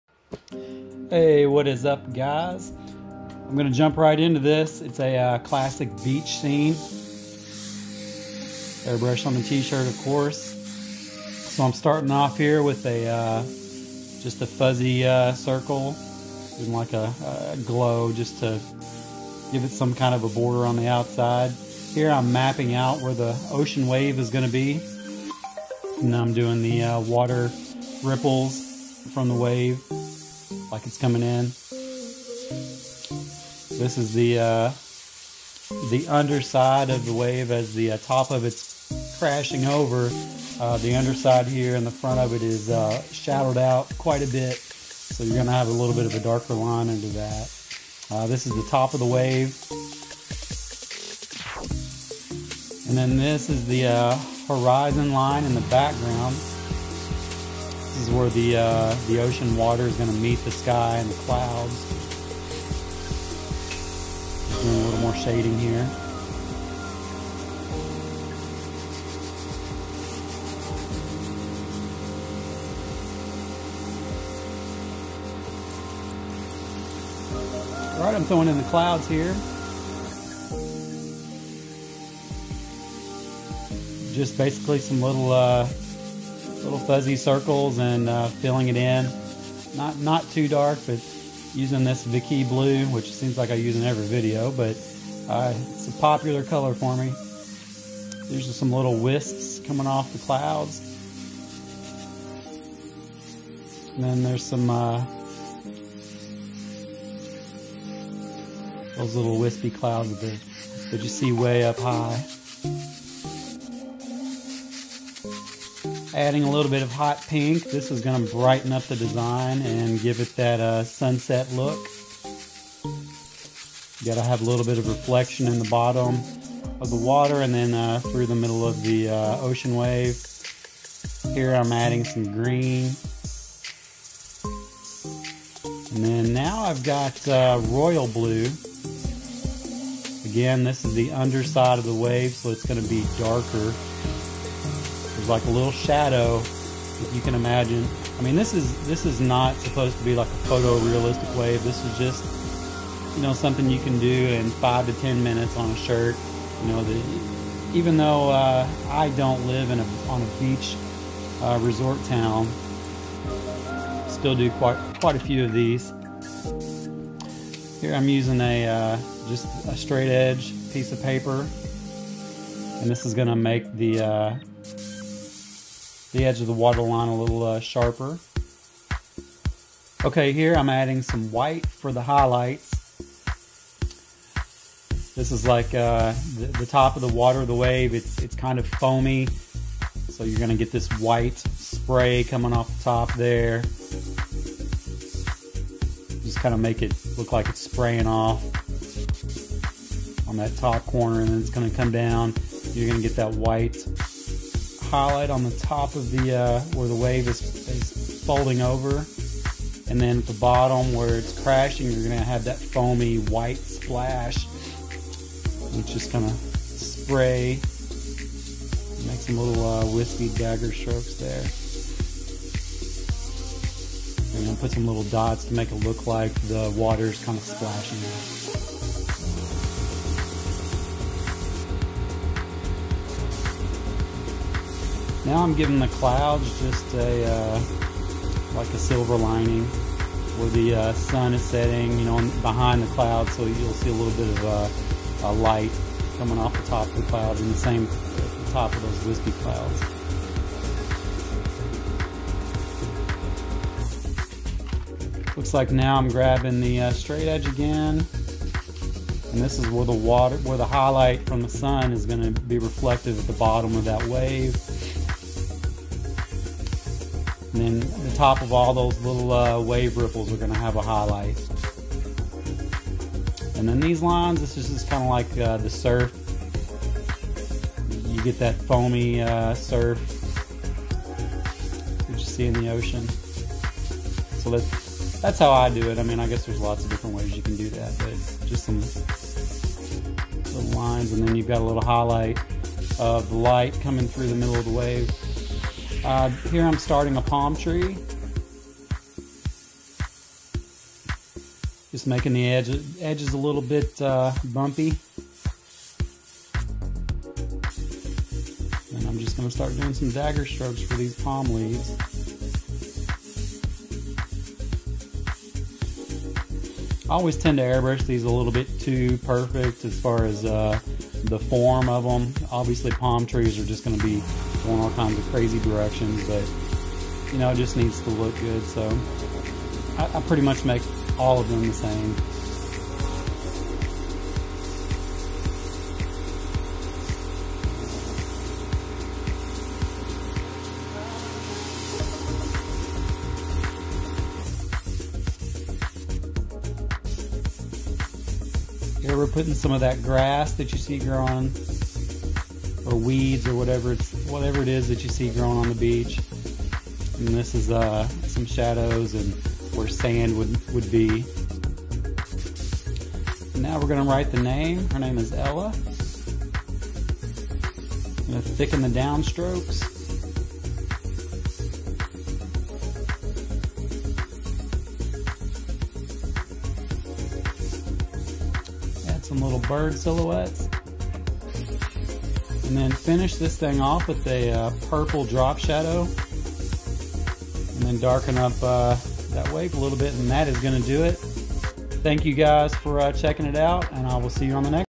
Airbrush beach ocean wave design.
In this video I'm demonstrating how to airbrush a ocean wave design on a t-shirt.